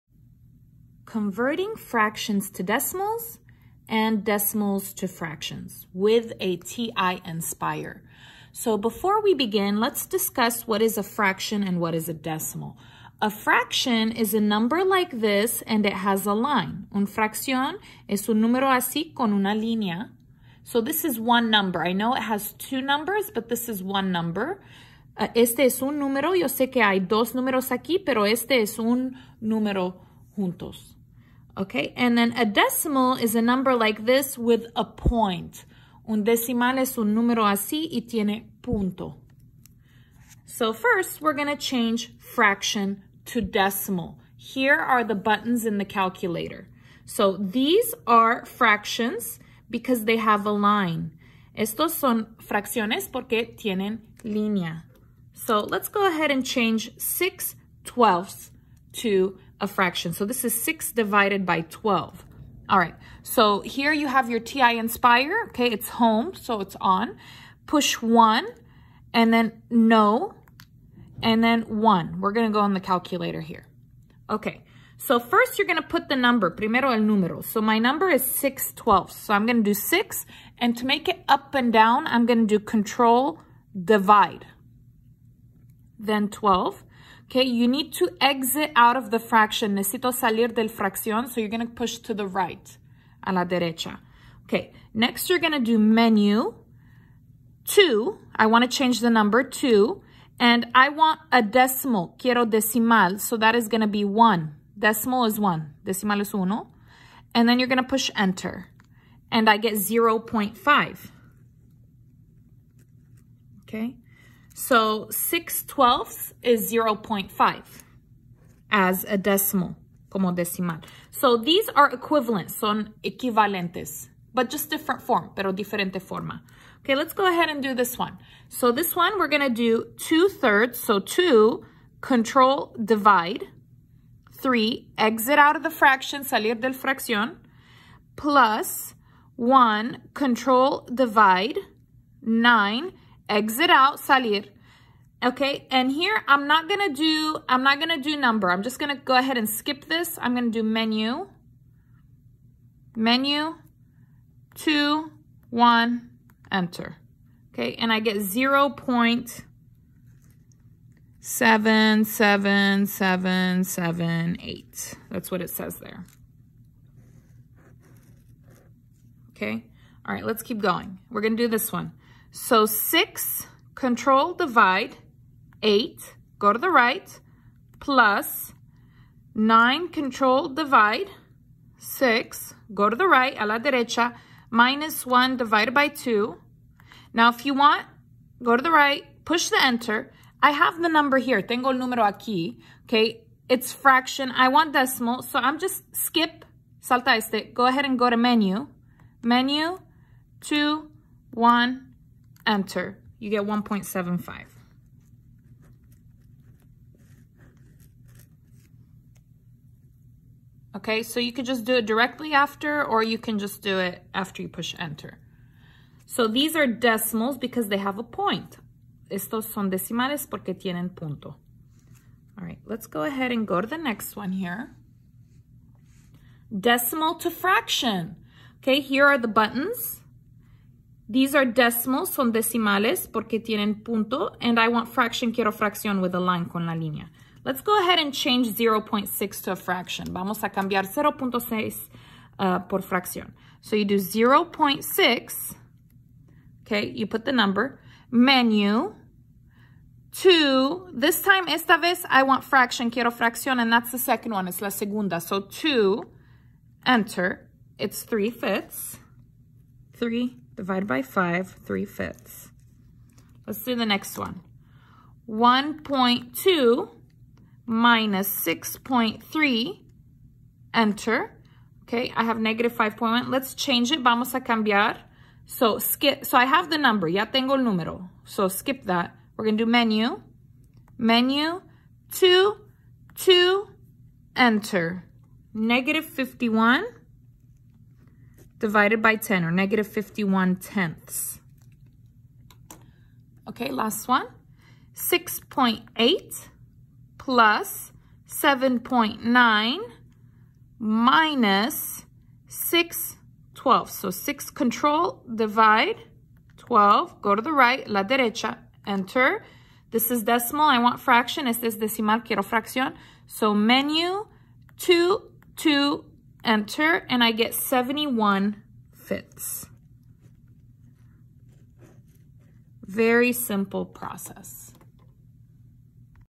**EASY CHEAT!** Converting from Fraction to Decimal and Back with a TI-Nspire
Convert fractions to decimals and vice versa using only your TI-Nspire calculator. It's so EASY!